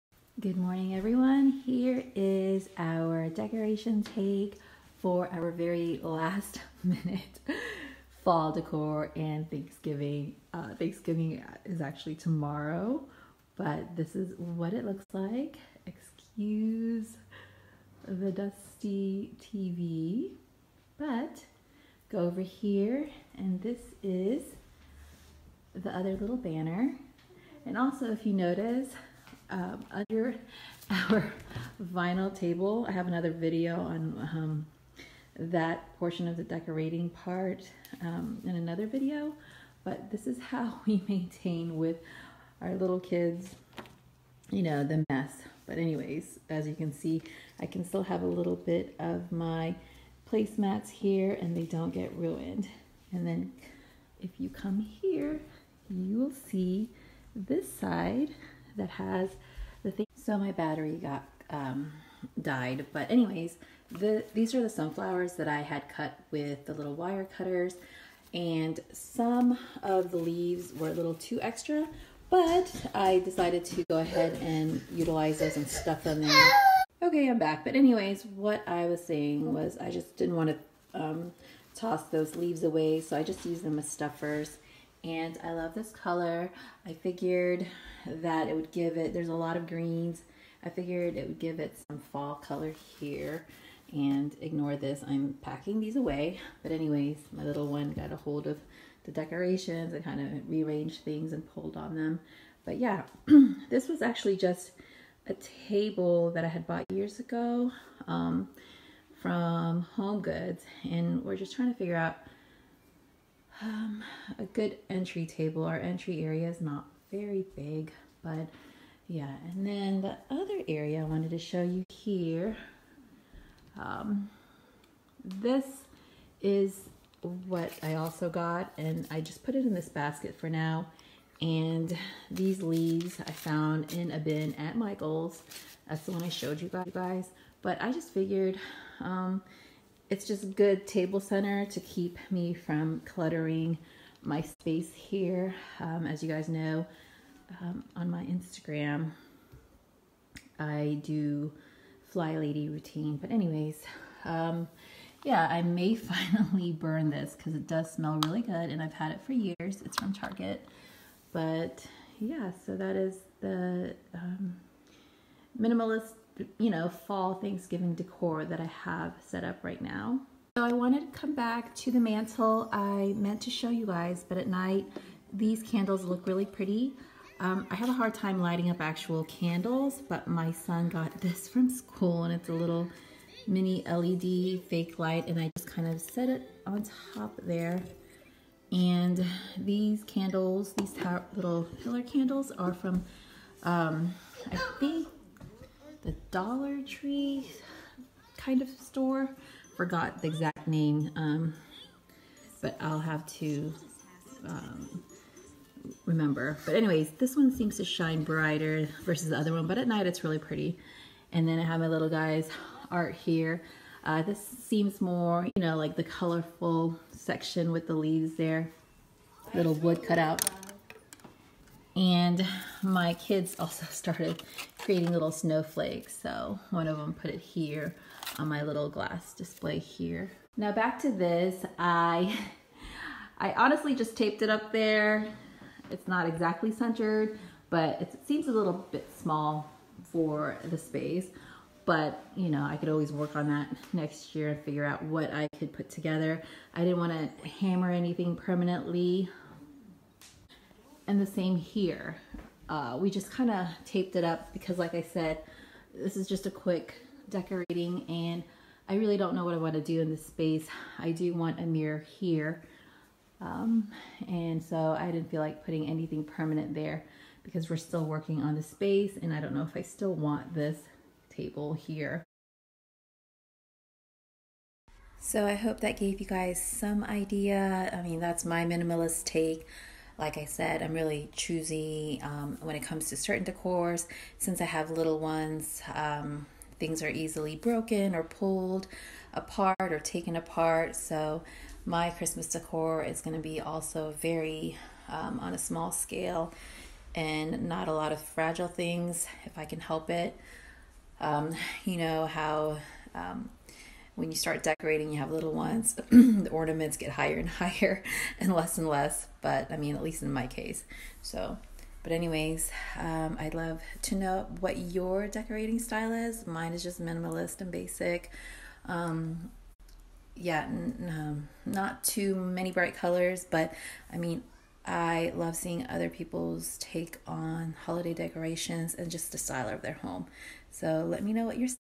Fall Decor Haul 2021- Thanksgiving Decorating Idea- Part 2 of 2
Late Fall/Thanksgiving decorator here BUT I can be choosy about holiday decor pieces and their sizes 🍁🏺 AND I love the SALES to splurge for them!
Michael’s craft store had cute, minimalist decorations at great sales prices!

 ➤ Rakuten Refer A Friend Code [formerly Ebates]
(You'll receive a $10 check back in the mail):